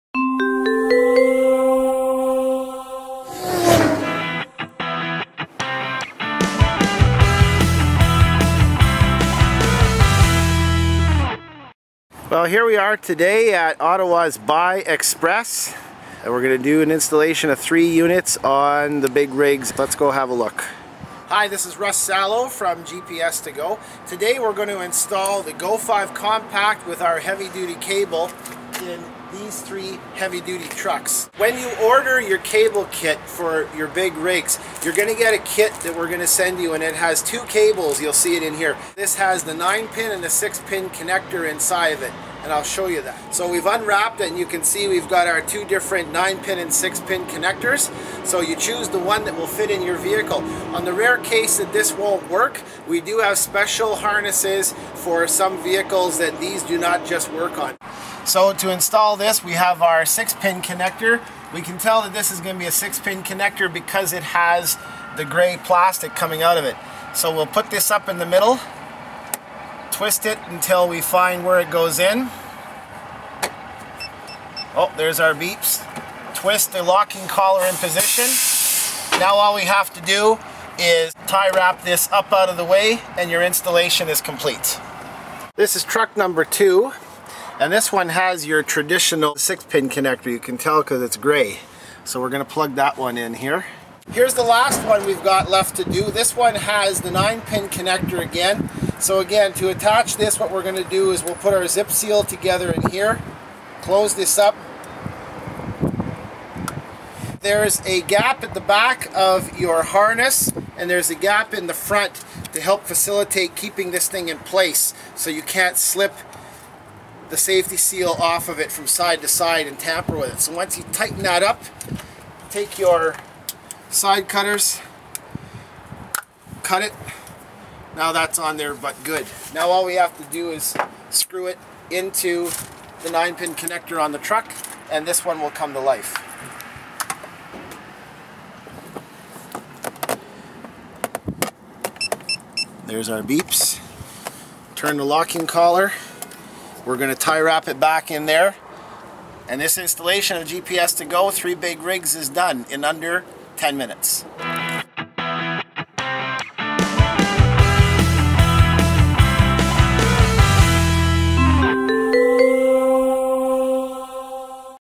GPS Installed on 3 BIG Rigs under 10 Minutes
GPS to GO President Russ Salo shows you how fast you can install the GO5 Compact unit from Geotab. In under 10 minutes we installed 3 complete GPS Tracking systems into these 3 Big Tractor Trailers and were visible on the Internet Tracking website.